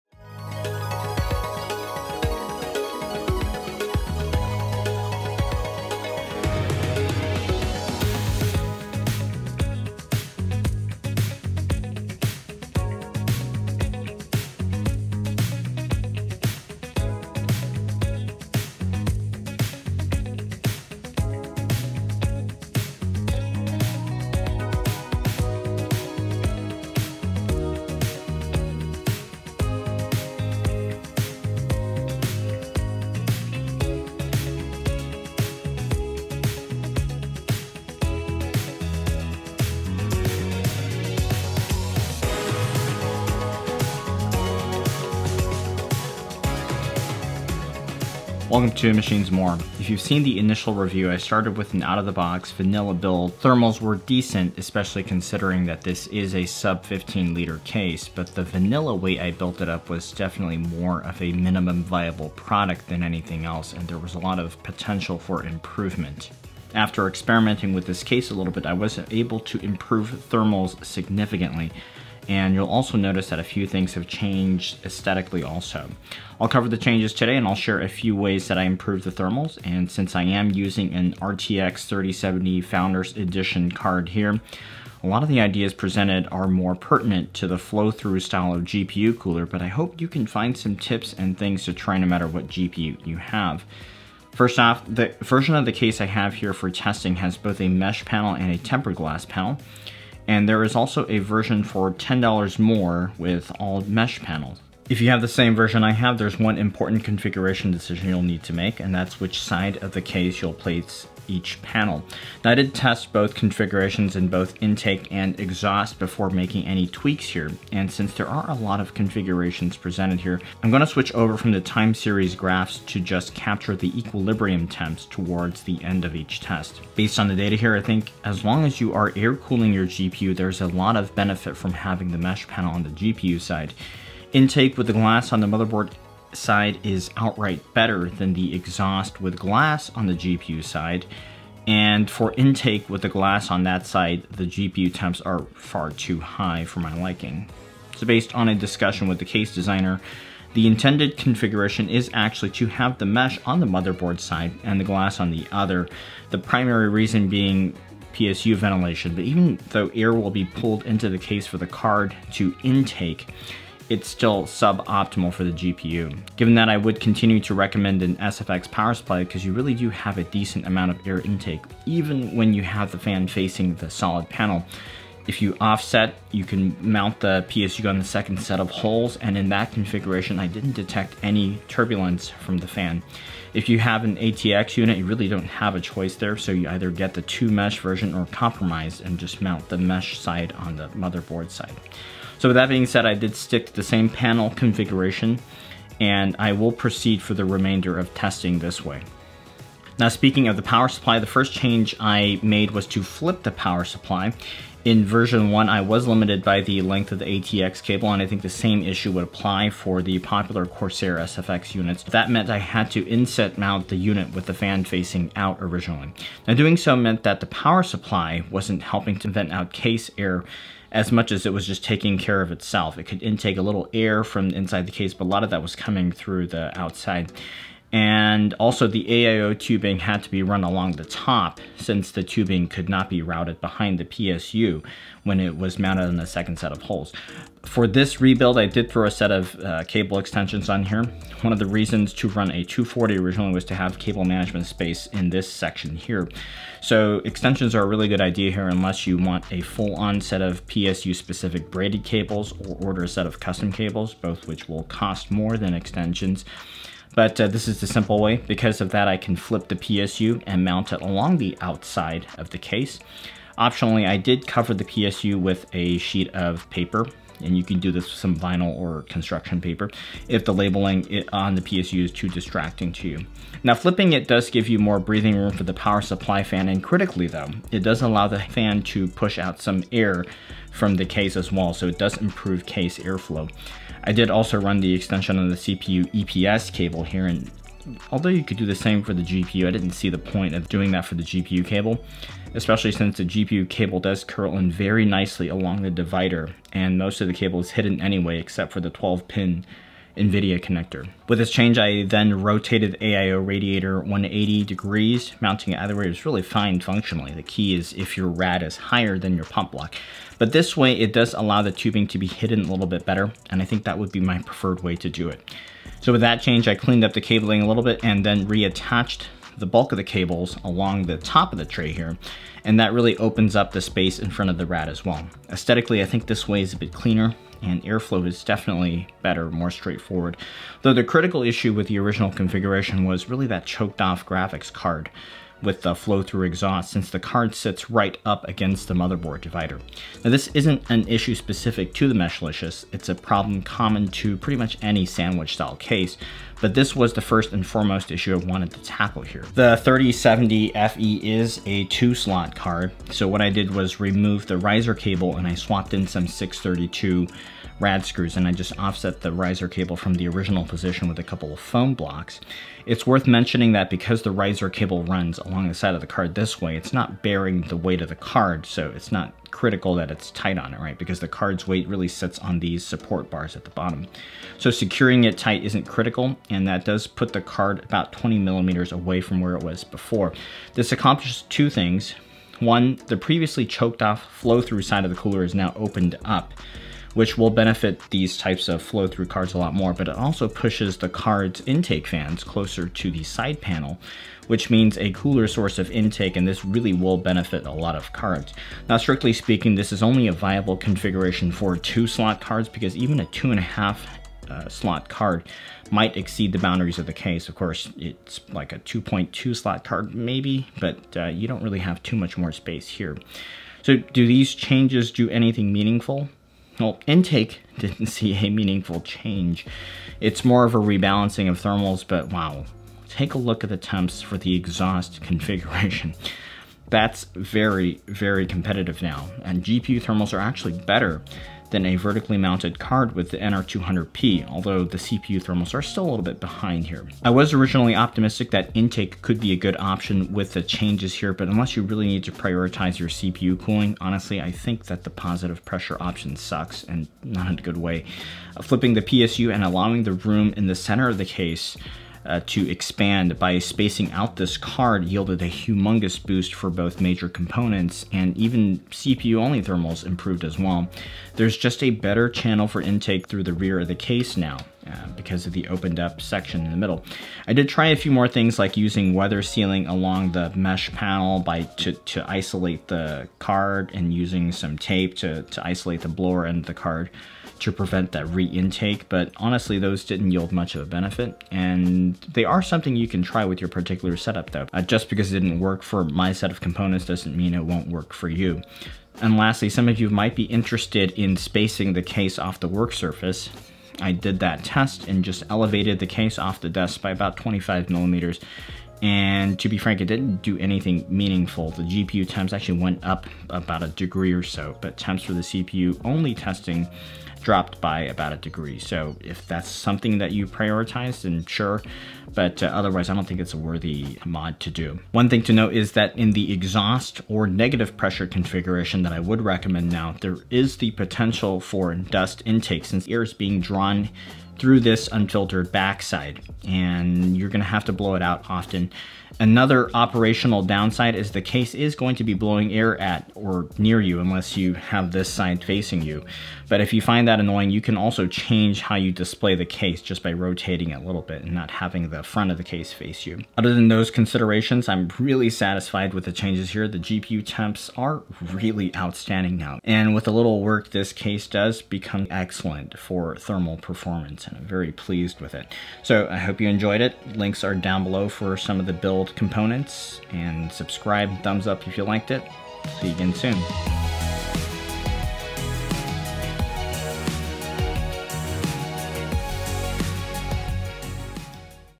SSUPD Meshlicious Ep. 2 : Airflow and Thermal Improvements
In ep. 2 of this SSUPD Meshlicious series, I aim to resolve some of the airflow issues that held back the thermal performance of the initial build. Where should you place the tempered glass panel? Which airflow configuration is best with an AIO? And that pesky flow through GPU? These answers and more, in this fact finding mission.

Extensions-
8 + 8 pin GPU
8+ 6 pin GPU

Screws - I used 30mm 6-32 rad screws, longer stand-offs are also OK too but you will need 20mm longer than the originals.

Case - SSUPD Meshlicious
CPU Cooler
RGB Fans
Graphics Card - Nvidia RTX 3070 Founders Edition
M.2 Drive

0:00 Introduction
1:34 Tempered Glass and the Mesh Panel?
3:20 Build updates - PSU Orientation and AIO Tubes
5:30 Flow Through GPUs...
6:53 Any thermal improvement?
8:08 Raising the case?